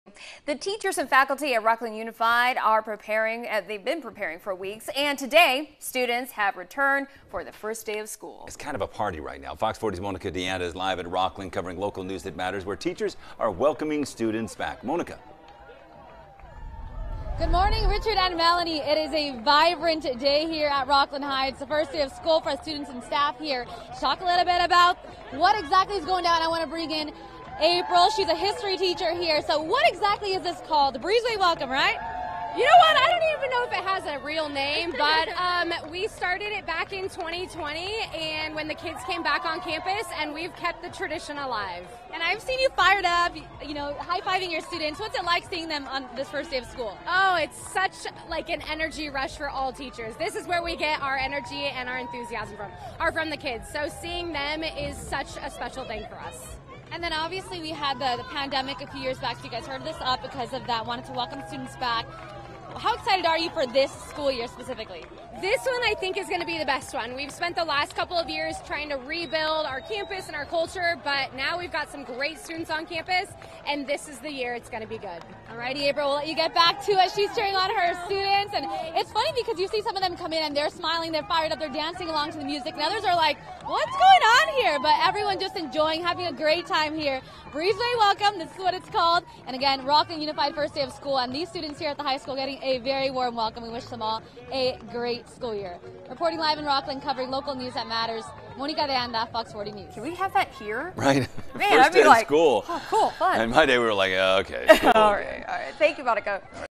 Teachers and Faculty of Rocklin Unified Celebrate the Return of Their Students
Since 2020, the teachers and faculty of Rocklin Unified have been cheering on their students as they return to campus for the first day of school.